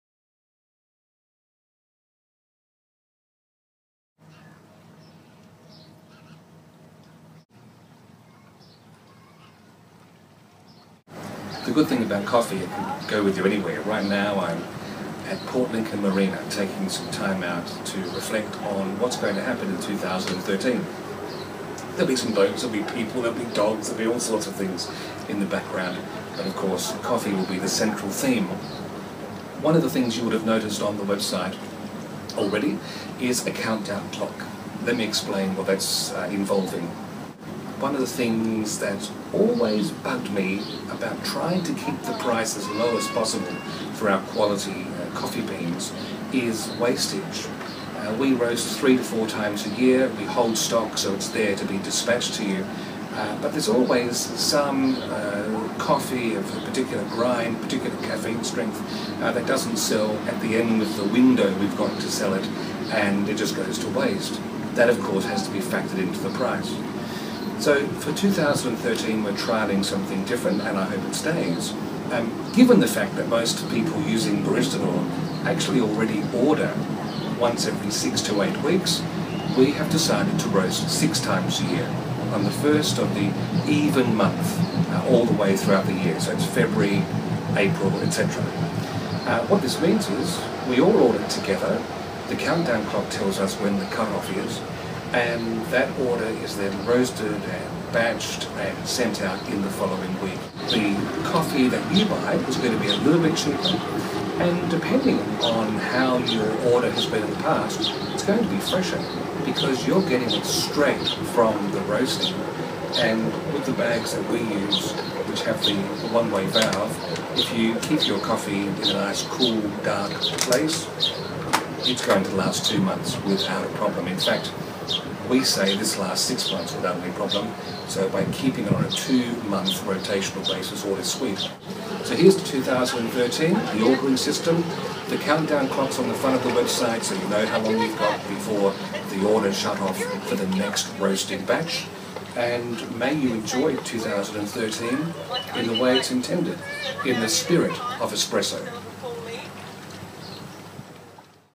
New ordering system at Baristador Coffee
This is a quick video for Baristadorians about our new approach to ordering. We are doing batch orders 6 times a year, rather than hold stock. This means we have been able to shave the price down (due to zero waste) and you get fresher coffee.